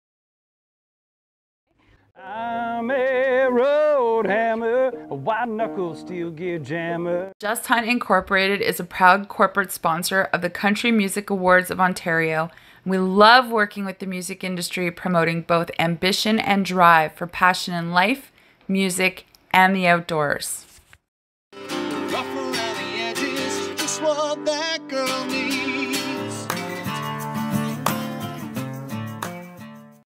CMAO COMMERCIAL for Just Hunt
Just Hunt INC., is a PROUD Corporate Sponsor of the CMAO and we love working with the music industry promoting both ambition and drive for passion in life, music and the outdoors.

The CMAOntario Awards bring emerging artists to the forefront and celebrate excellence and outstanding professional achievement by Ontario country music artists and songwriters. This inimitable live event features appearances and performances by Ontario’s brightest talents.

CMAOntario Awards Show:
Sunday, September 5 from 7:00-9:30pm
Ancaster Fairgrounds
630 Trinity Road South